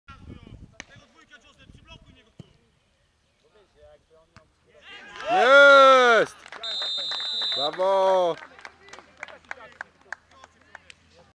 Norbert Stróżyk - Spójnia II - Odra Pławidło
Gol Norberta Stróżyka w meczu Spójnia II - Odra Pławidło